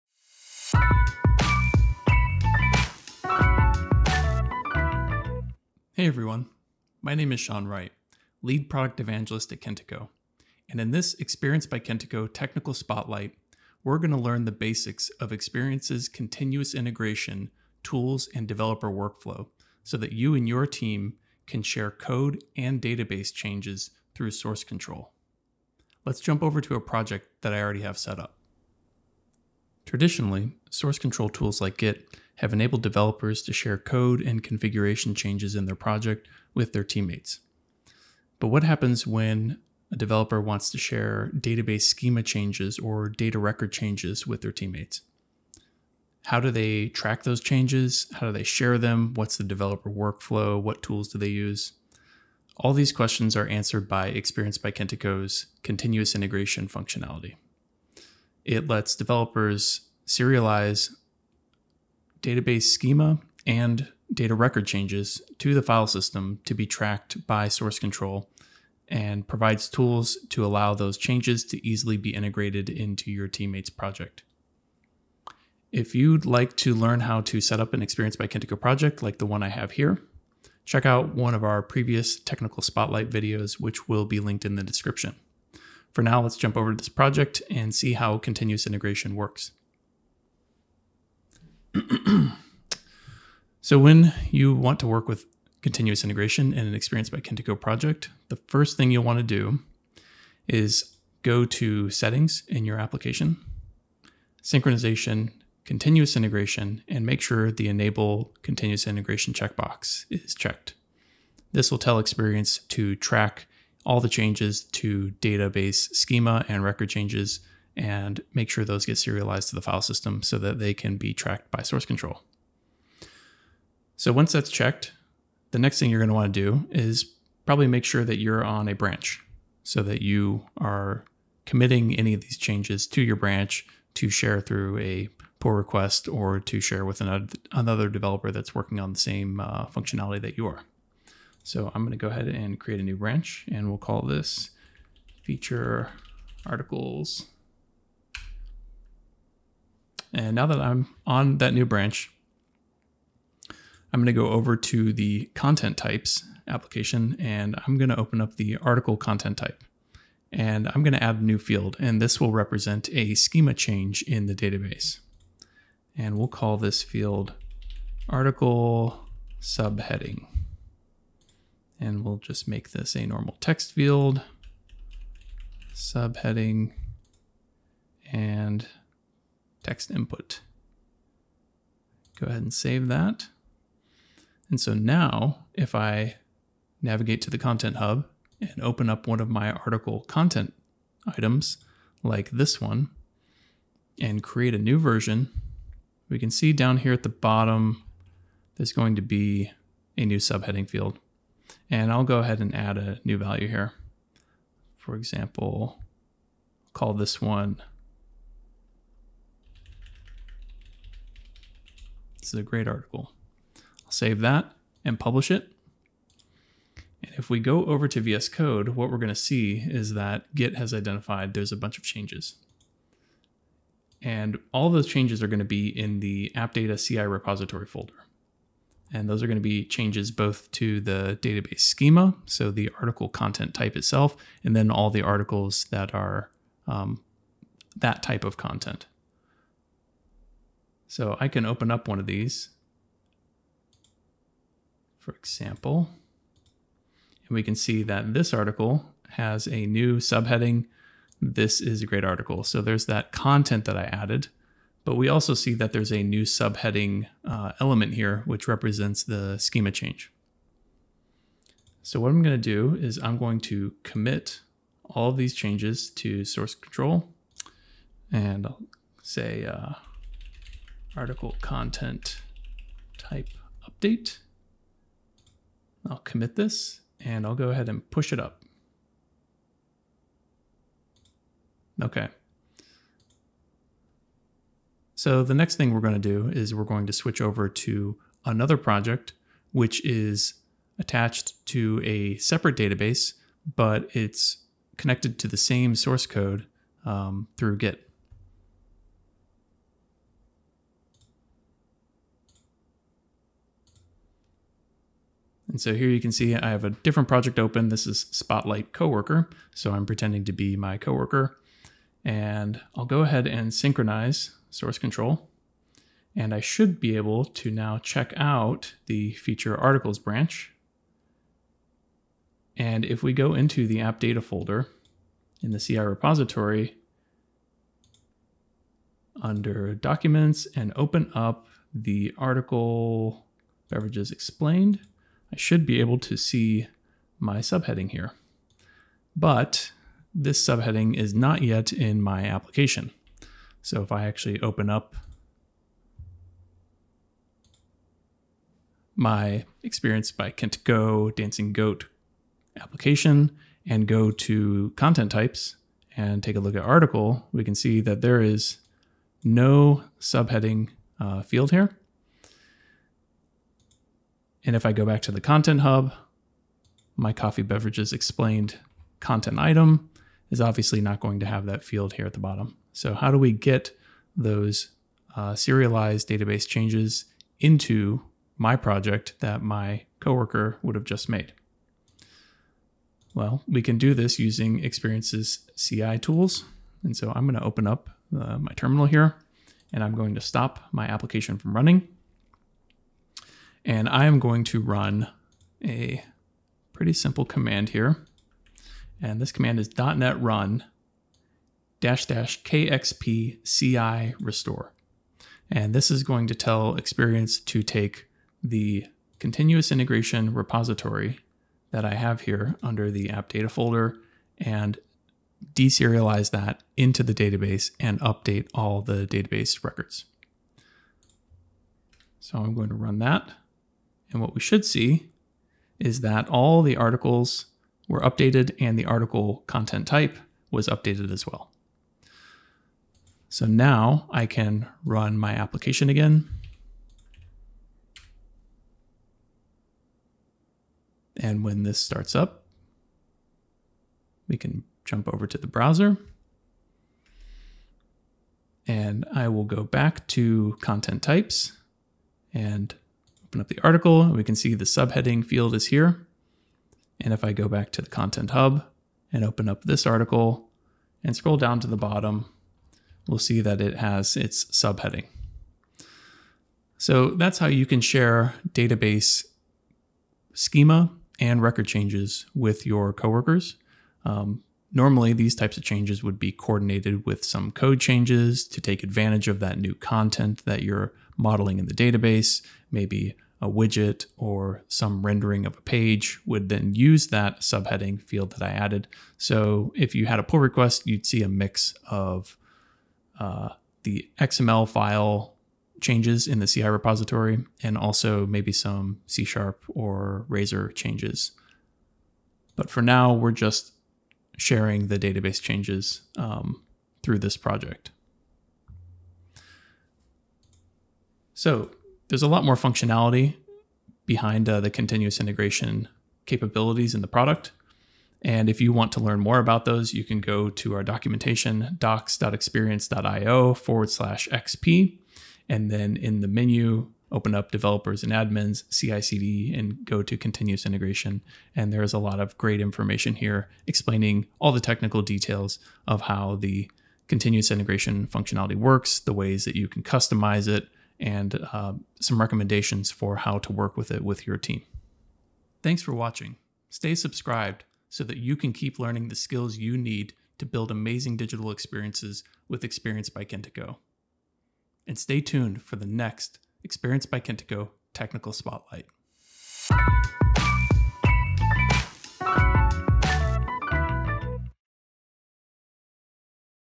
Technical Spotlight #3: continuous integration developer workflow
Developers know how to use source control to track changes to code and configuration, but how do they track database schema and data changes? How can they share these changes and collaborate with their team? Xperience by Kentico answers these questions with its powerful Continuous Integration tools and capabilities.

To learn how to setup a new Xperience by Kentico sample site from scratch, watch our Getting Started with Xperience by Kentico video.